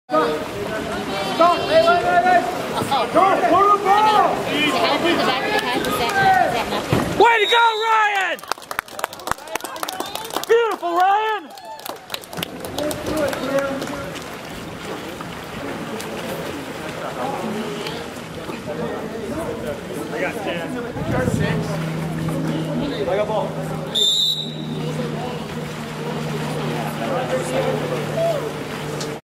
Ryan H Chadwick School water polo goal vs. Webb 10- 22-10 in HD
Ryan H of Chadwick picks up a loose ball and slams it into the net to help Chadwick to a victory over Webb on 10-22-10 at Chadwick.